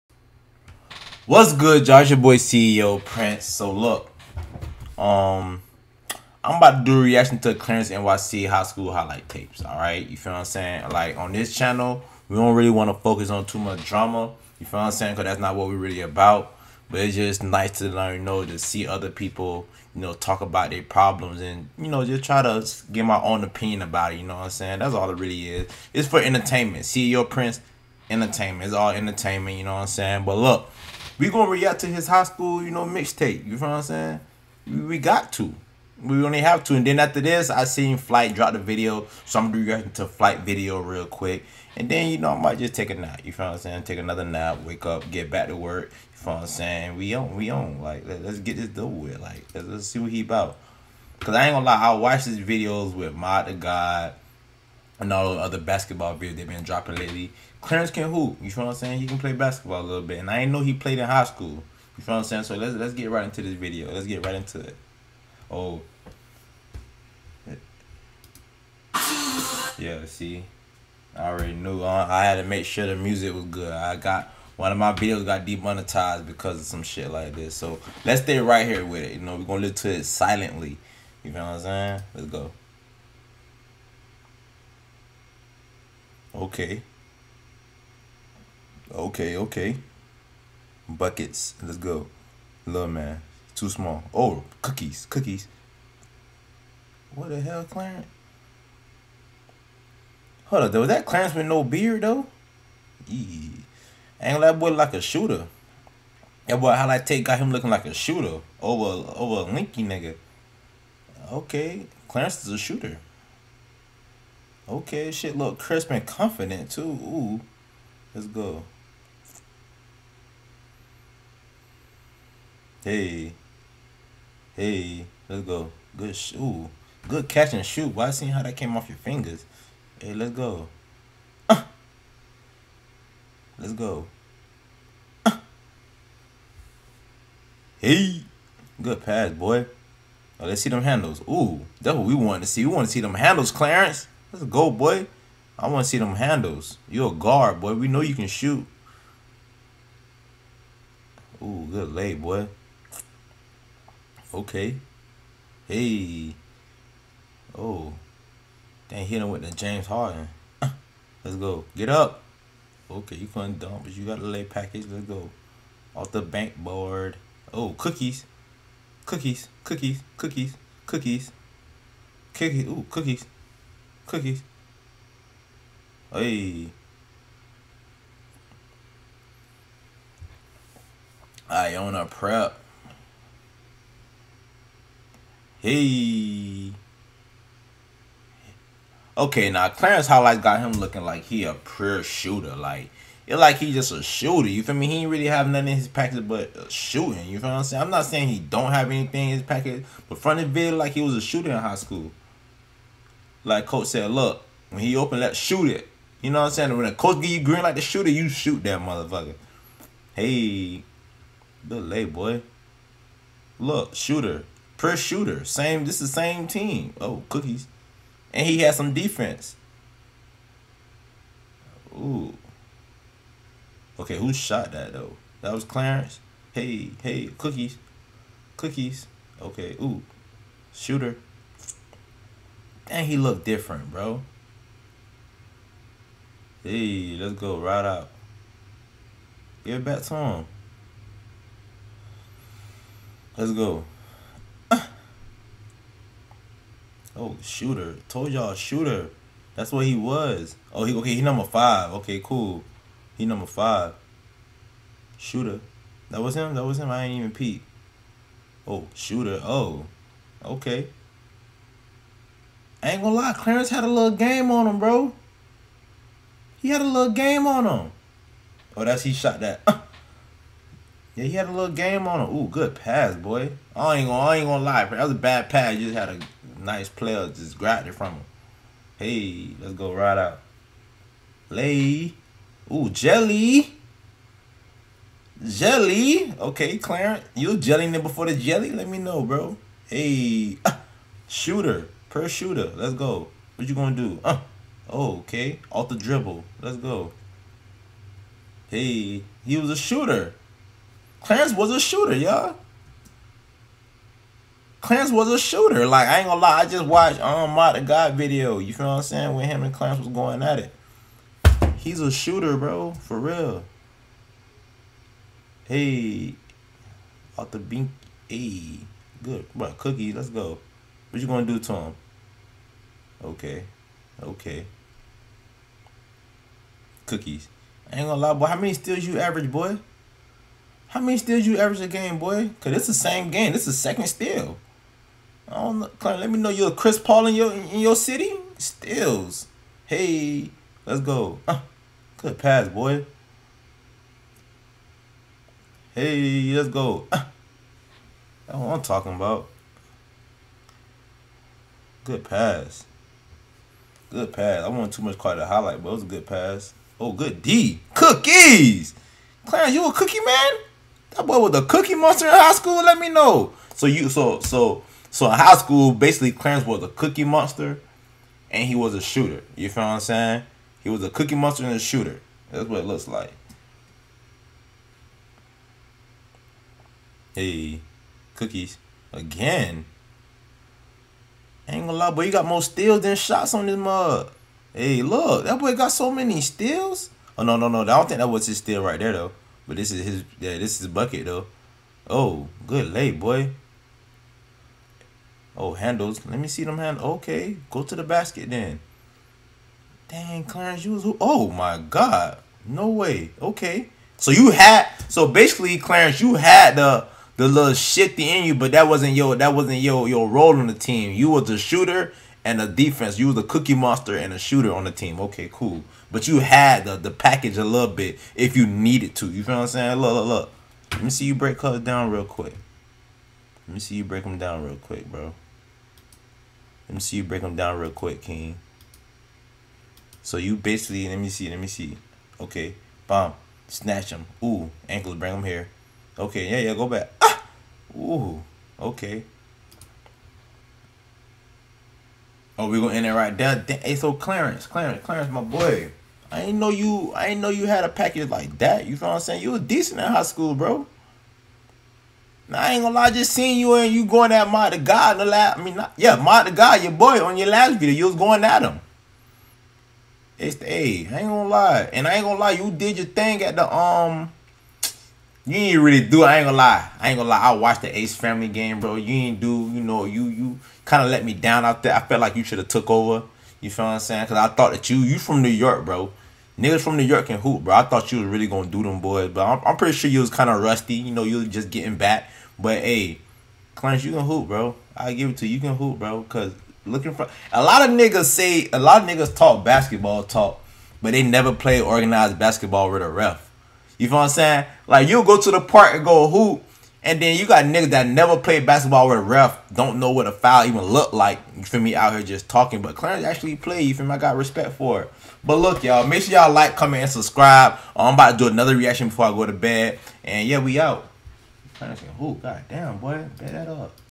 My Reaction to ClarenceNYC High School Basketball Highlights
Clarence was a Shooter and Cookie Monster in High School, I want to see ModdaGod Highlights.